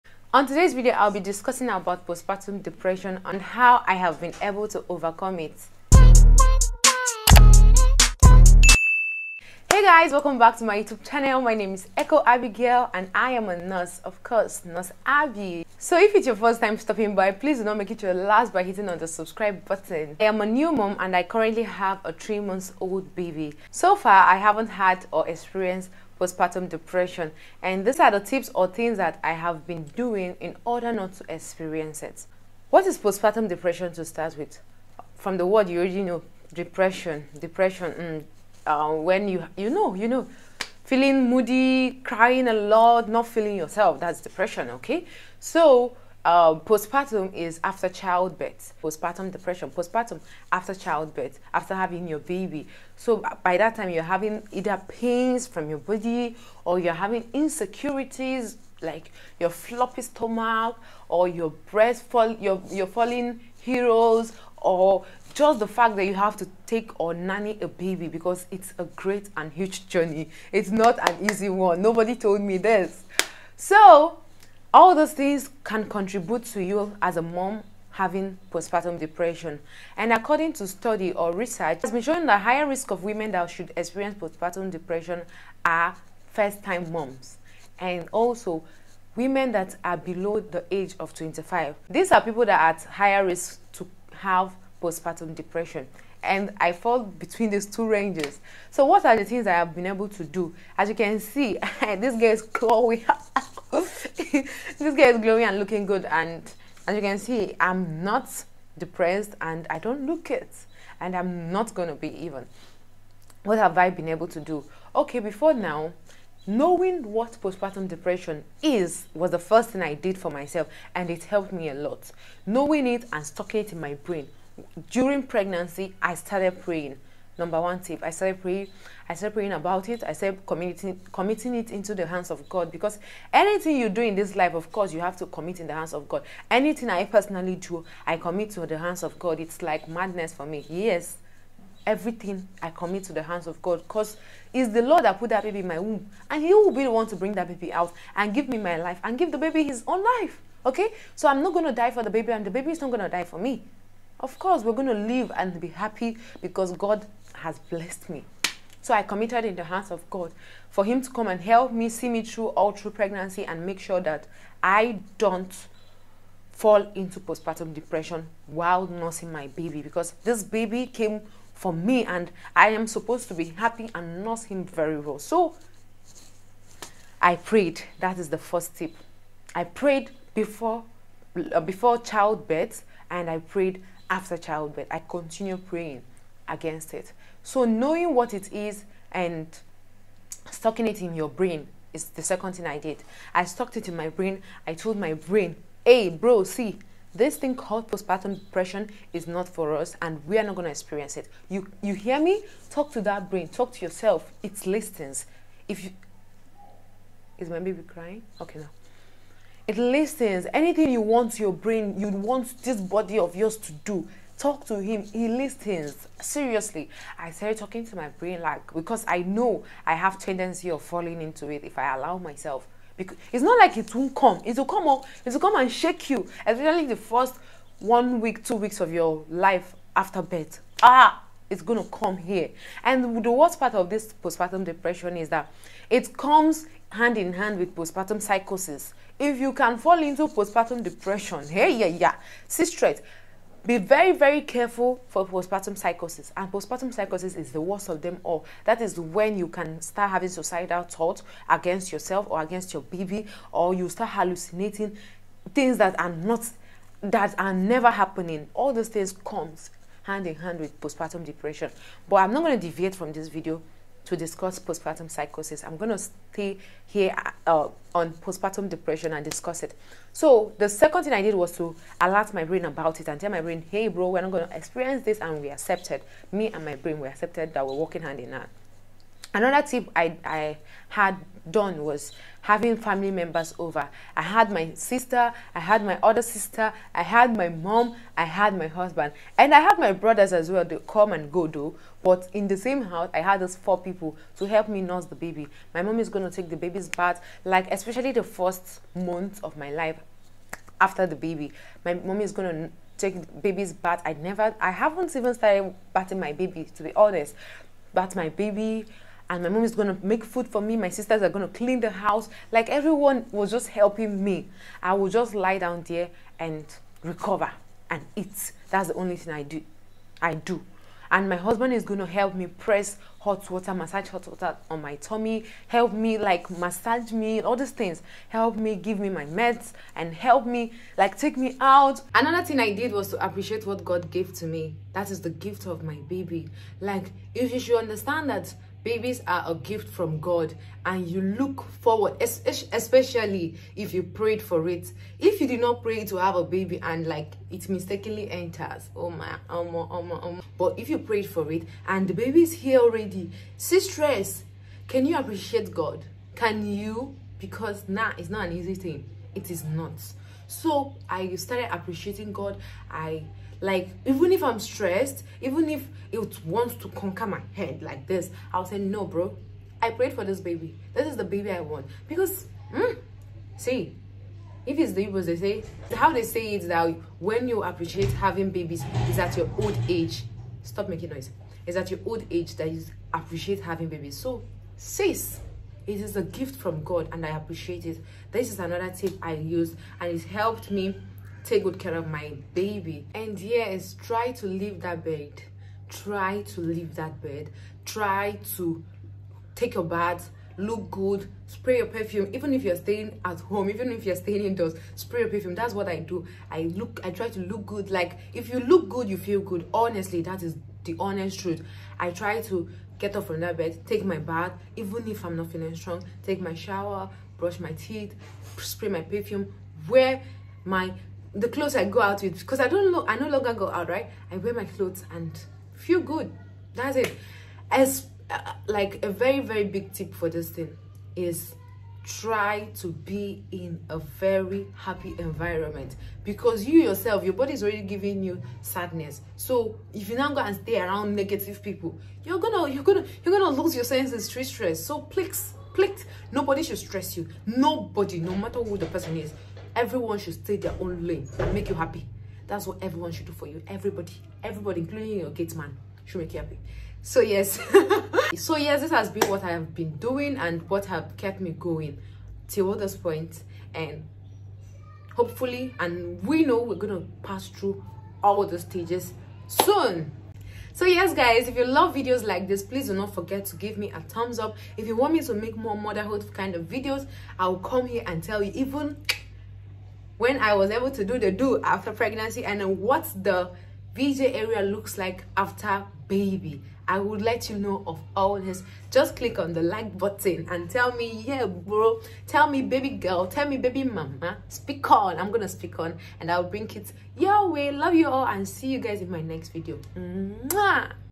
POSTPARTUM DEPRESSION
As a new 20+ mum, in this video I discussed about postpartum depression and how to overcome it.. How I overcome postpartum Depression as a first time mom.

Subs Count-3634